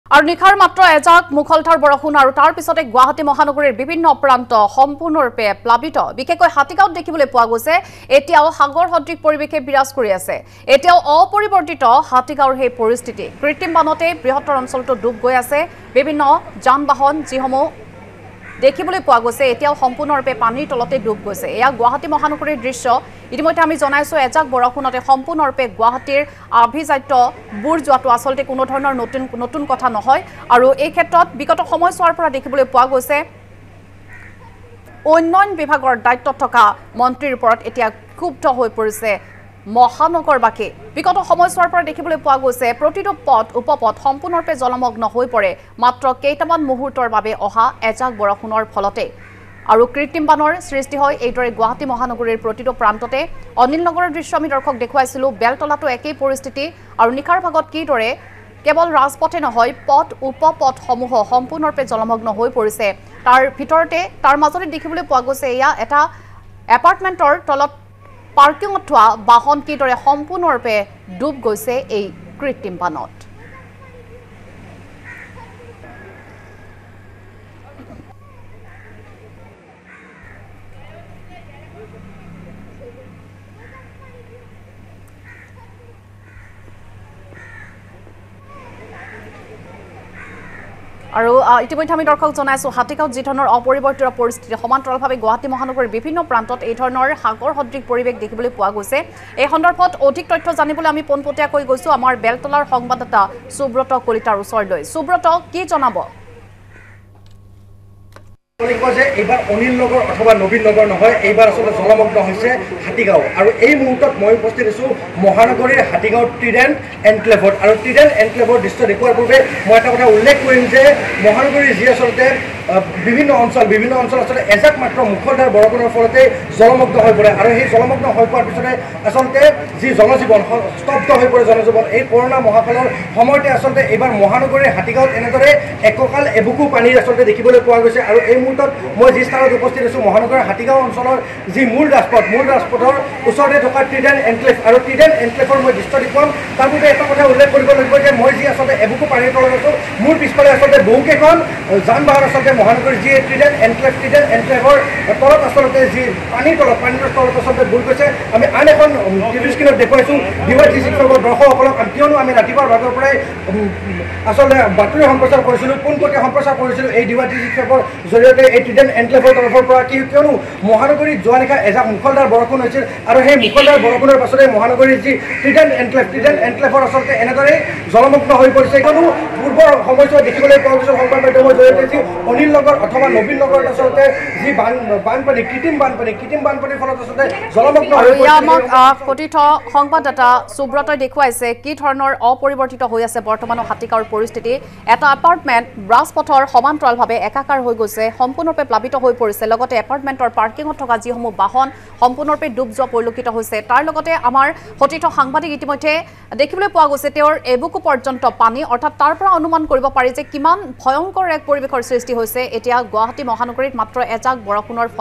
Flash flood in Hatigaon, Guwahati | নিশাৰ ধাৰাষাৰ বৰষুণত হাতীগাঁৱত সাগৰসদৃশ পৰিৱেশ
- নিশাৰ ধাৰাষাৰ বৰষুণত হাতীগাঁৱত সাগৰসদৃশ পৰিৱেশ
- মঙলবাৰেও অপৰিৱৰ্তিত হাতীগাঁৱৰ পৰিস্থিতি
- কৃত্ৰিম বানত ডুবি আছে বৃহত্তৰ অঞ্চলটো